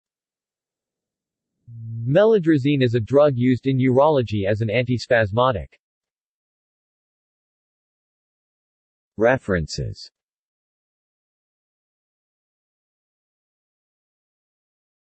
Meladrazine | Wikipedia audio article
This is an audio version of the Wikipedia Article:
Meladrazine

SUMMARY
Meladrazine is a drug used in urology as an antispasmodic.